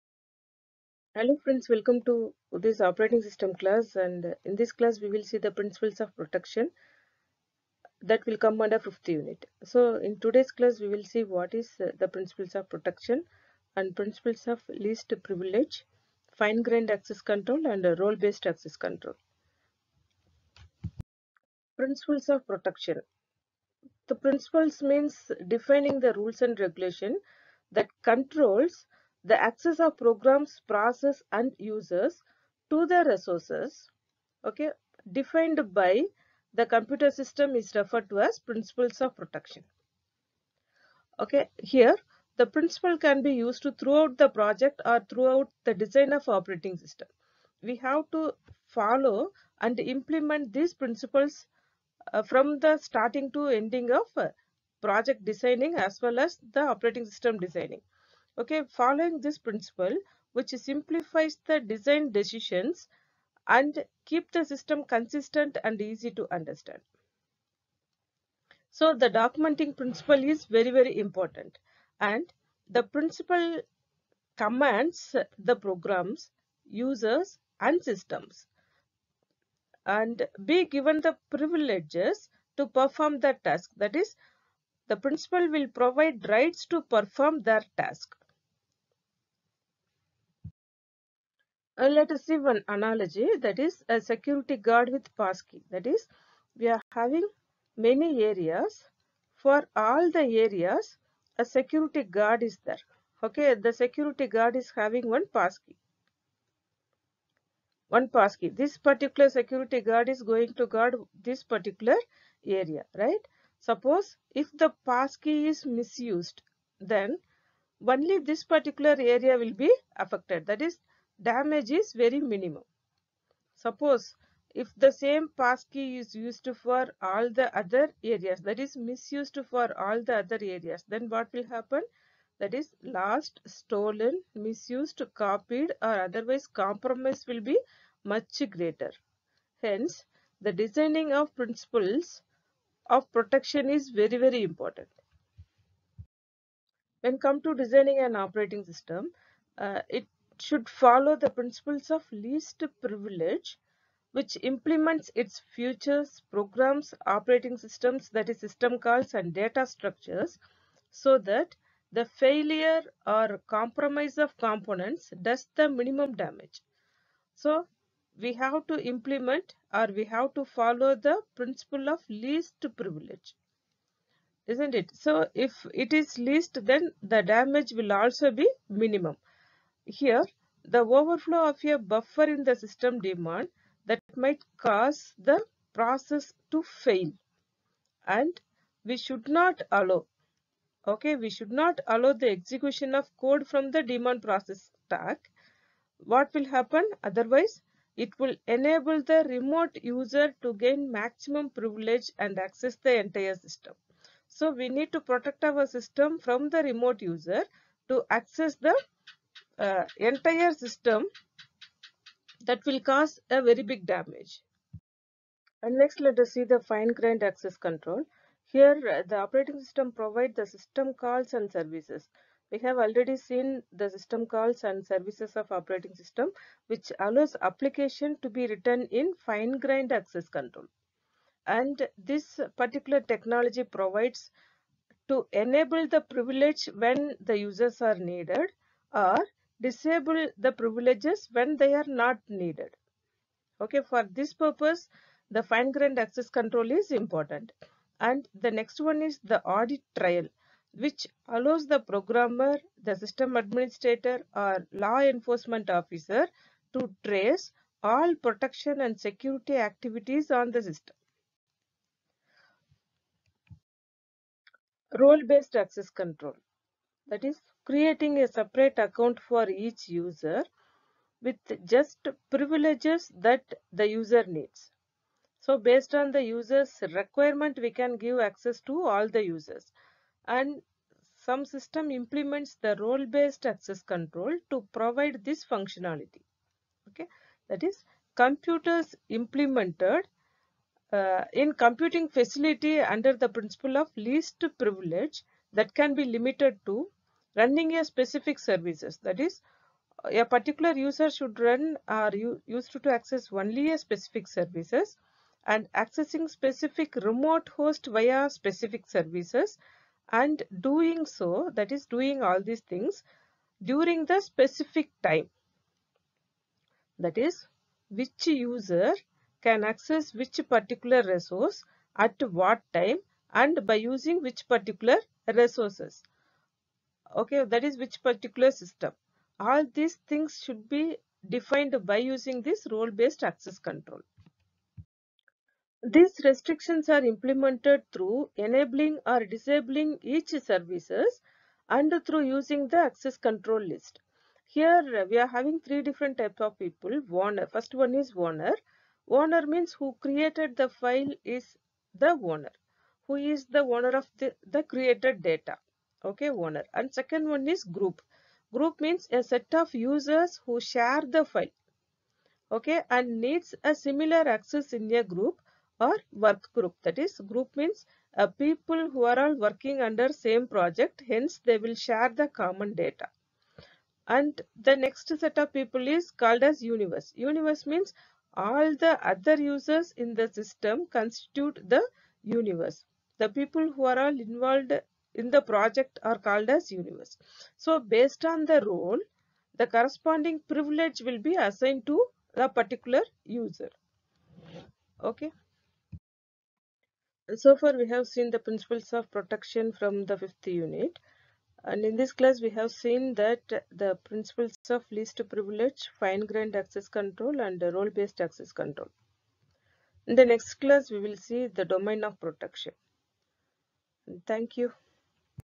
Principles of Protection-Operating Systems-20A05402T-UNIT – 5 Protection and System Security
UNIT – 5  Protection and System Security
Principles of Protection

Principles of Least Privilege,
Fine-grained Access Controls,
Role-based Access Control (RBAC).

For Syllabus, Text Books, Materials and Previous University Question Papers and important questions